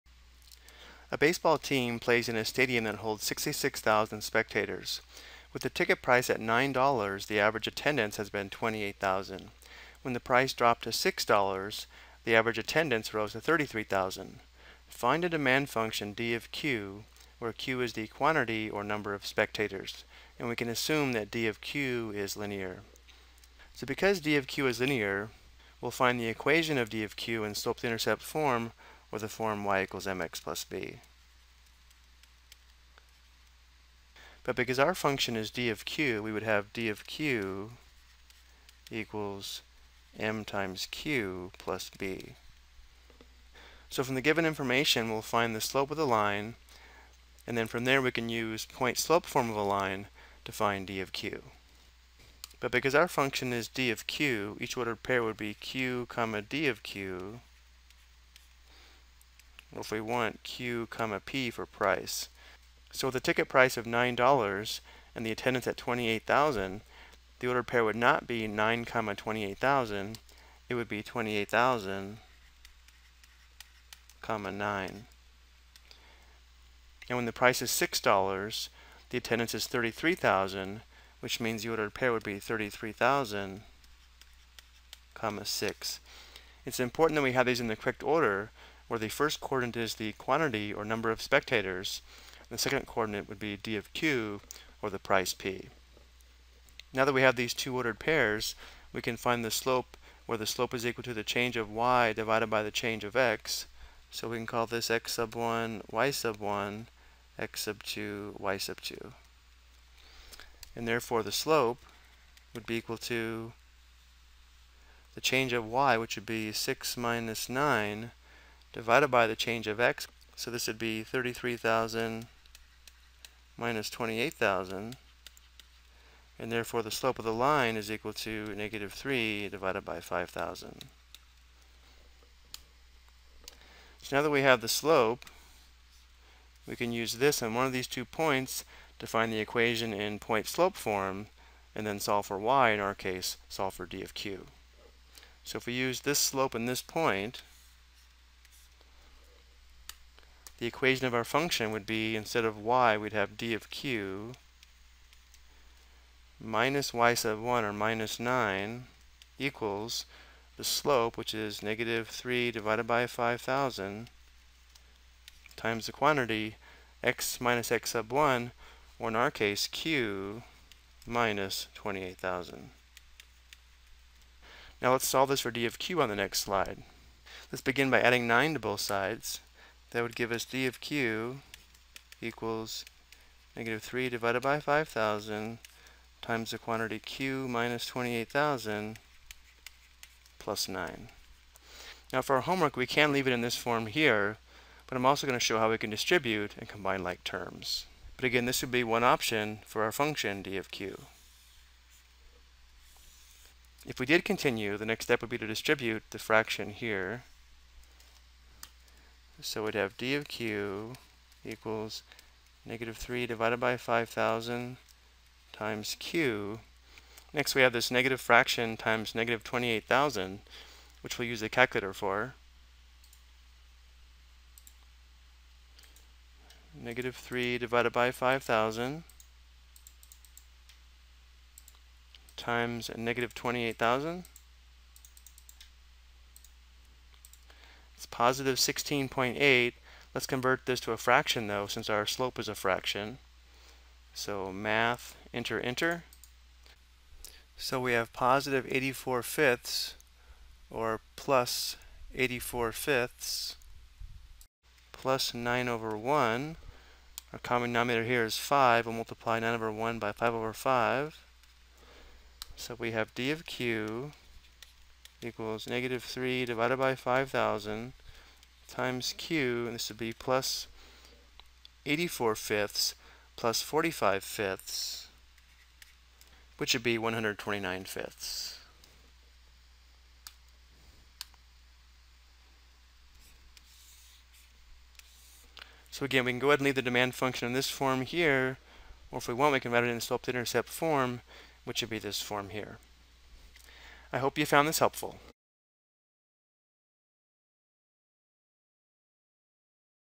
Ex: Determine a Linear Demand Function
This video provides an example of how to find a linear demand function from given information.